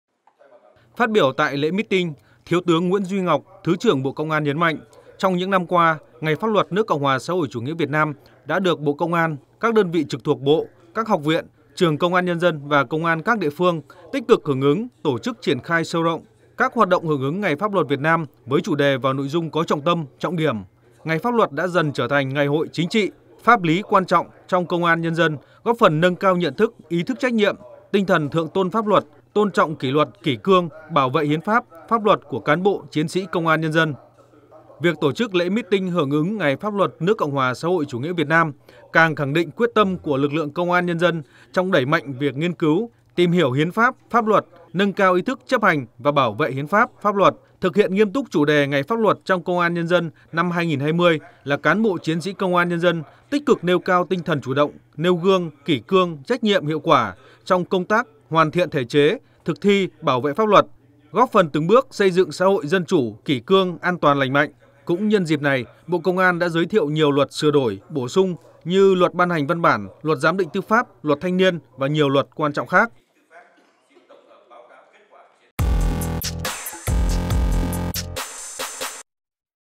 Bộ Công an kỷ niệm Ngày Pháp luật Việt Nam
Hưởng ứng Ngày Pháp luật Việt Nam 9/10, chiều 05/11, Bộ Công an tổ chức Lễ mít tinh hưởng ứng Ngày Pháp luật nước Cộng hòa xã hội chủ nghĩa Việt Nam năm 2020 và phổ biến một số luật được Quốc hội khóa XIV thông qua tại Kỳ họp thứ 9.